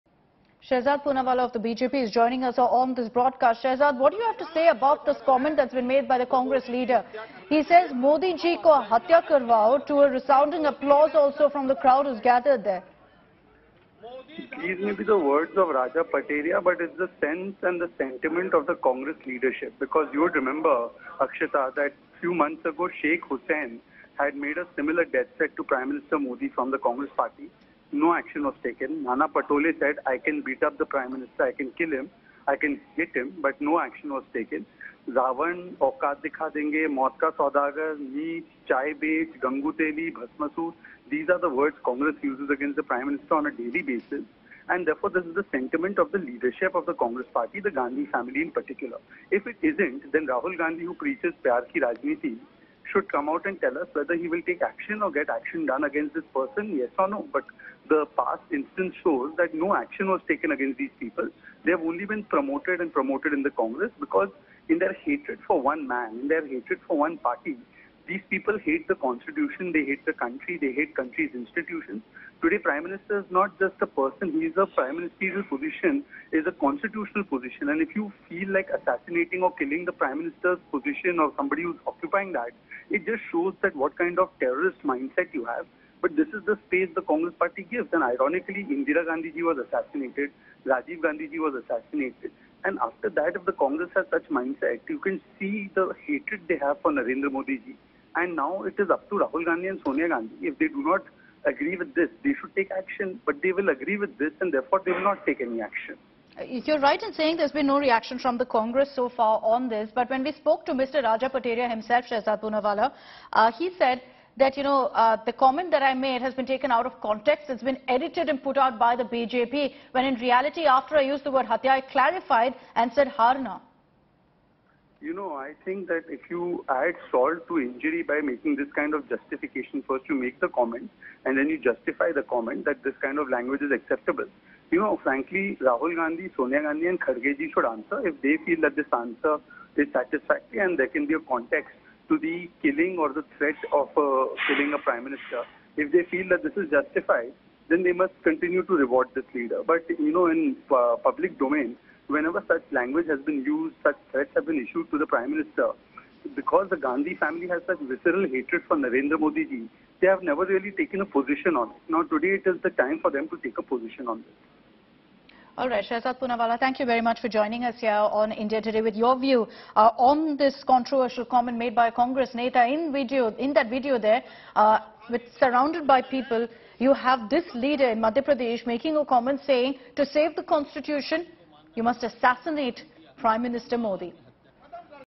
Congress Leader Raja Pateria 'Kill Modi' Remark Sparks Row, BJP Seek Action Against Him
Congress leader Raja Pateria, speaking to India Today, clarified that his remark against PM Modi was presented out of context. Raja Pateria said what he meant "by killing the PM" was to defeat Narendra Modi in the upcoming elections.

“It might have happened in the flow. But the person who recorded it just picked it out of context,” said the leader. In a video, the former Madhya Pradesh minister could be seen addressing his supporters around him.